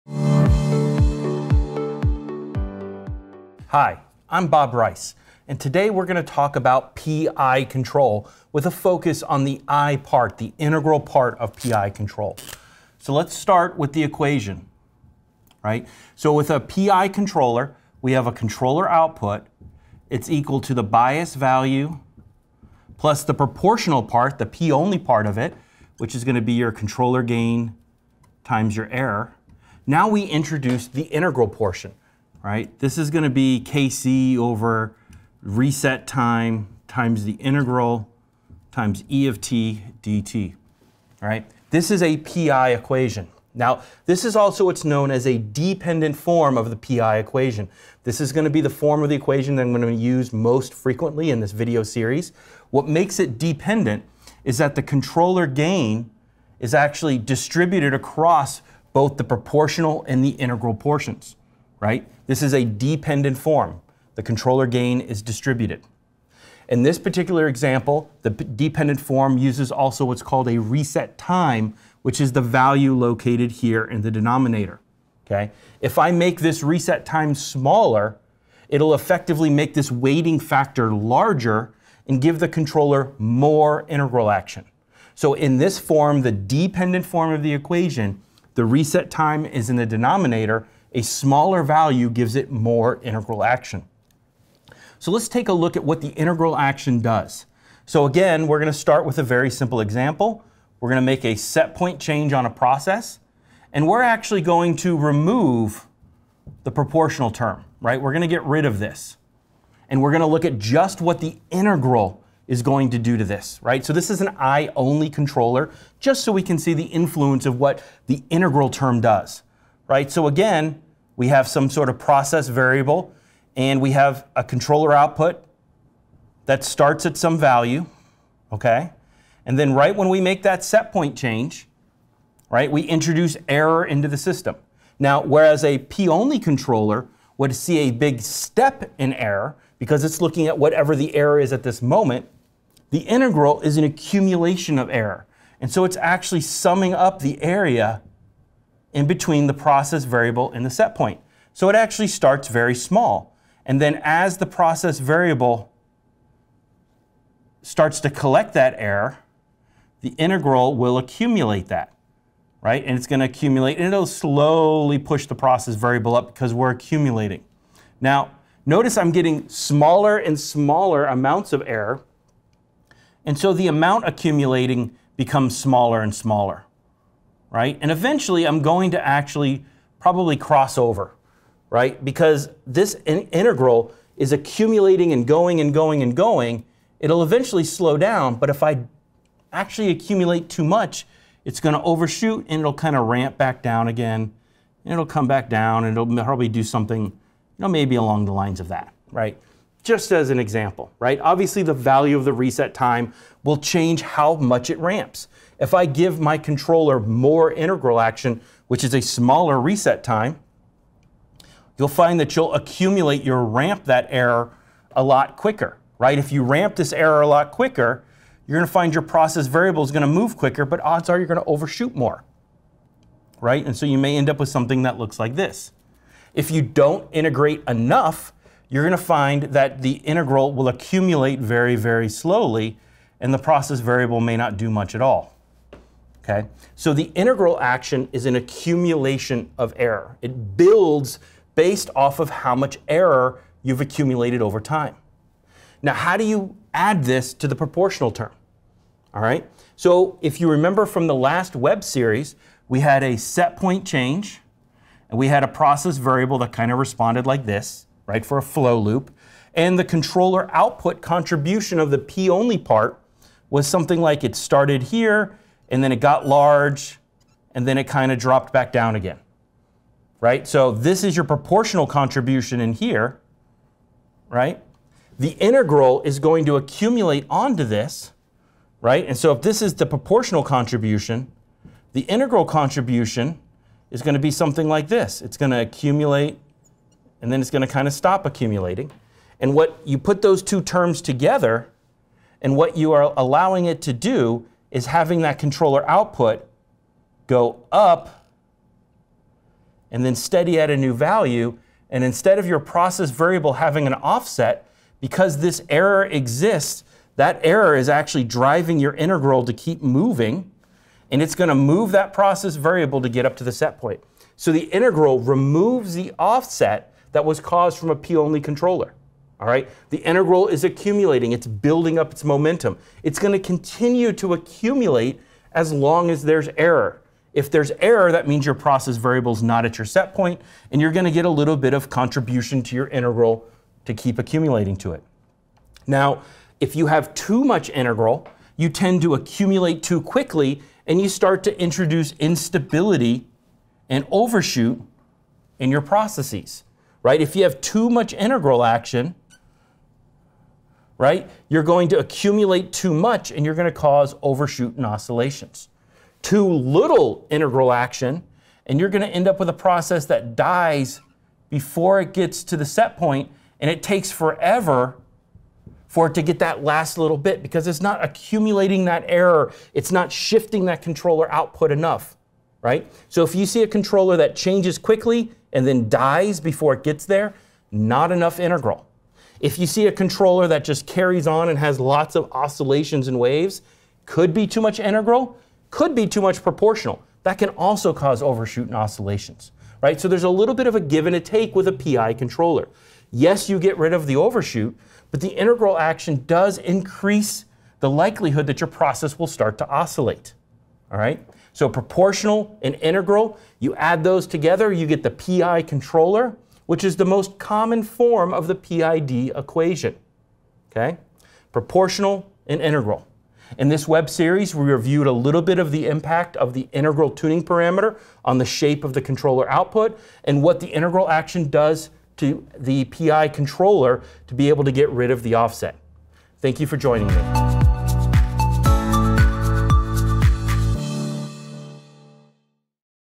Ask Dr Bob: What is PI Control?
Struggling with PID control? Looking for best practices? Need simple, understandable answers? Just Ask Bob.

Our next video series will feature Bob Rice. He'll provide no-nonsense answers to common Process Control and plant optimization challenges.